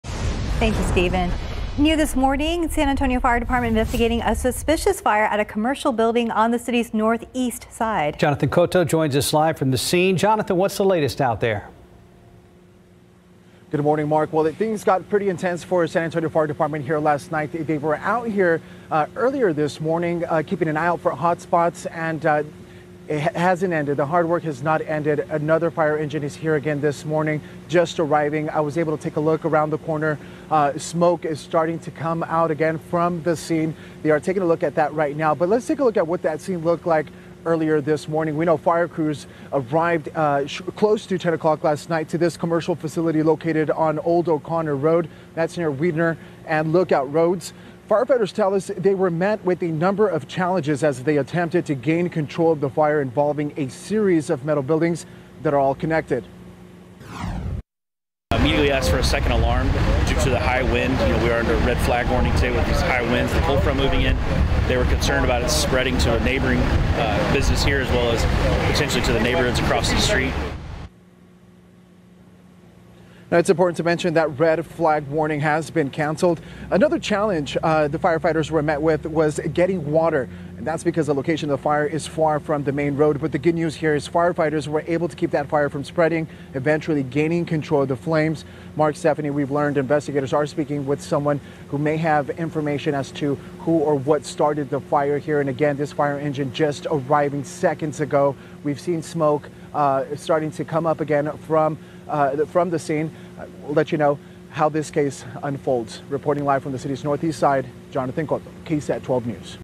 Suspicious fire at Northeast Side commercial facility prompts heavy response, SAFD says
Firefighters are working to extinguish a suspicious fire at a commercial facility on the Northeast Side, San Antonio Fire Department officials said.